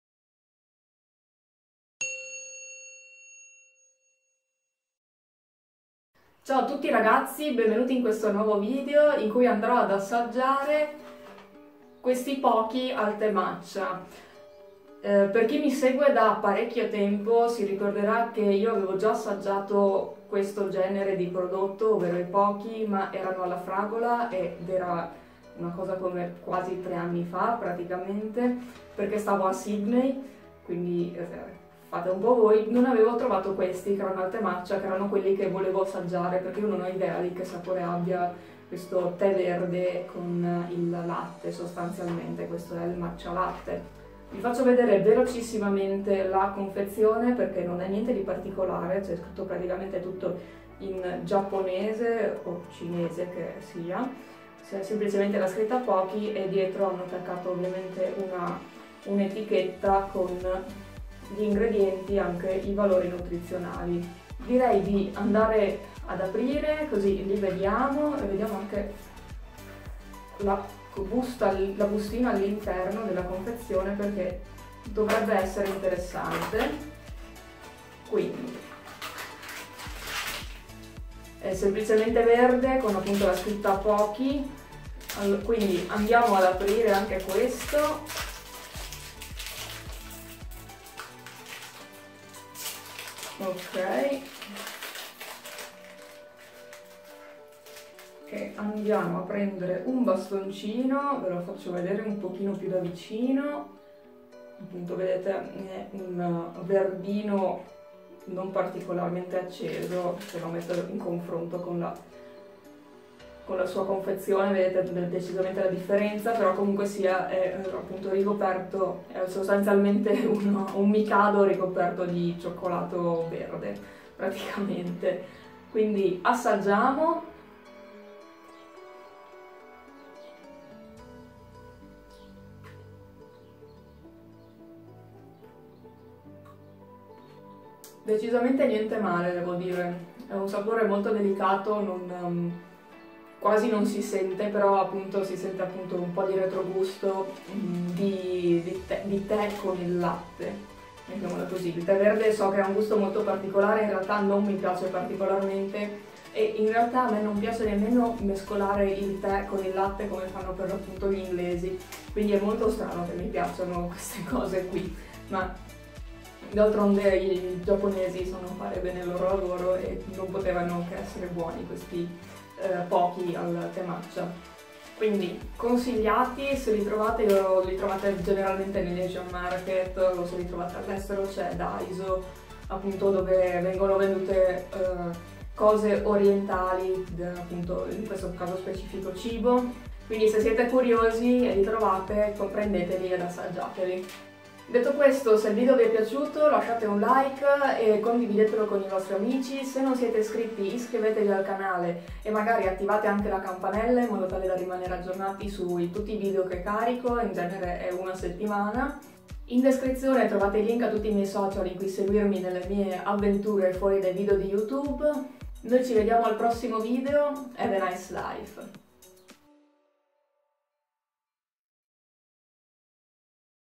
Pocky al matcha latte - assaggiAMO!
Dopo i Pocky alla fragola, è finalmente tempo di assaggiare i Pocky al matcha latte!